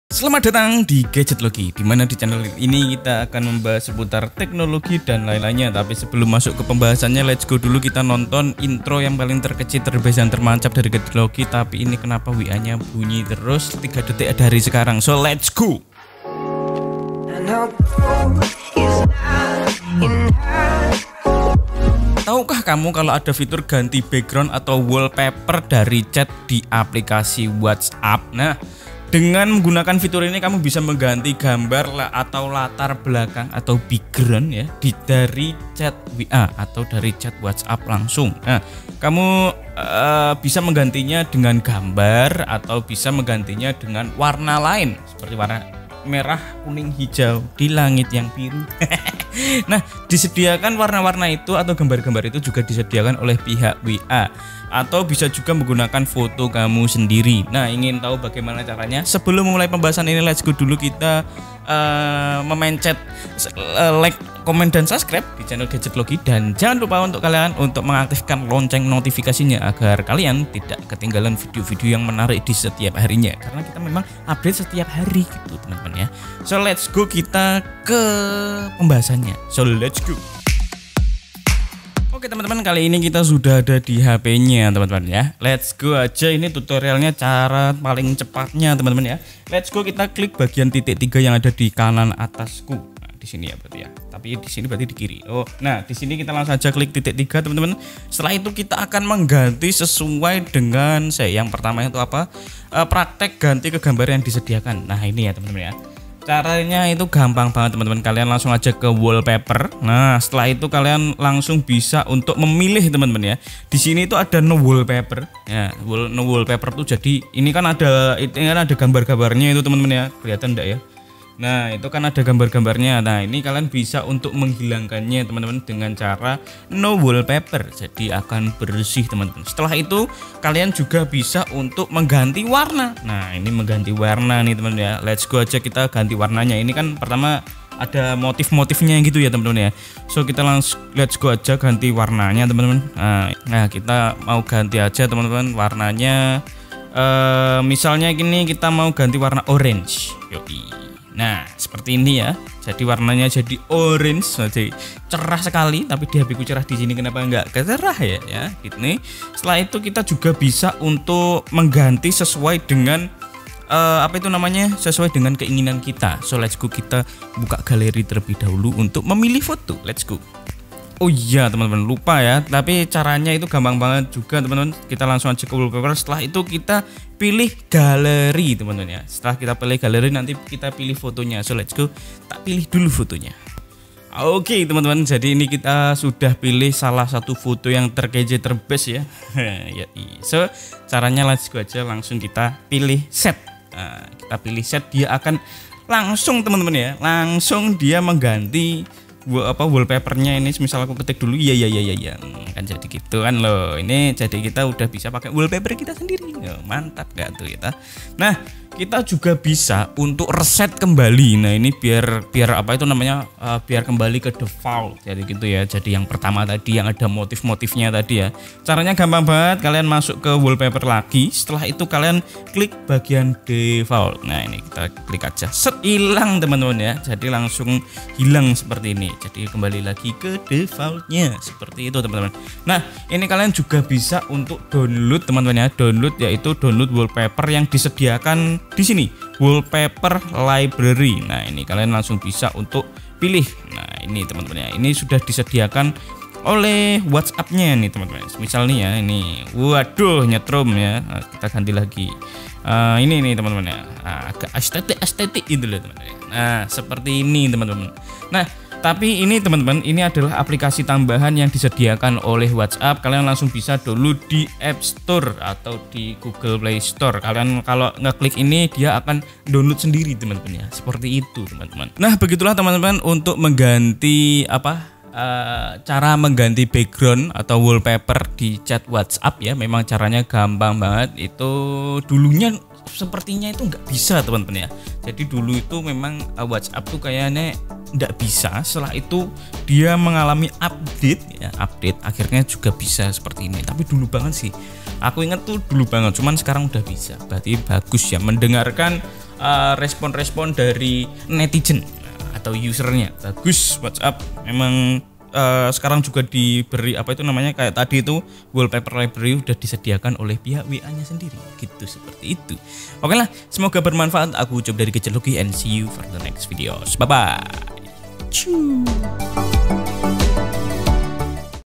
Cara Mengganti Background / Wallpaper Chat WhatsApp Versi Terbaru
🔥 Voucher Gratis Ongkir Shopee Hari Ini 🔥

Tahukah kamu kalau ada fitur ganti background / wallpaper dari chat di aplikasi WhatsApp?

Dengan menggunakan fitur ini, kamu bisa mengganti gambar latar belakang dari chat di WA kamu dengan gambar atau warna lain yang telah disediakan pihak WA atau bisa juga menggunakan foto kamu sendiri.

Nah, ingin tahu bagaimana caranya? tonton video ini sampai selesai, ya.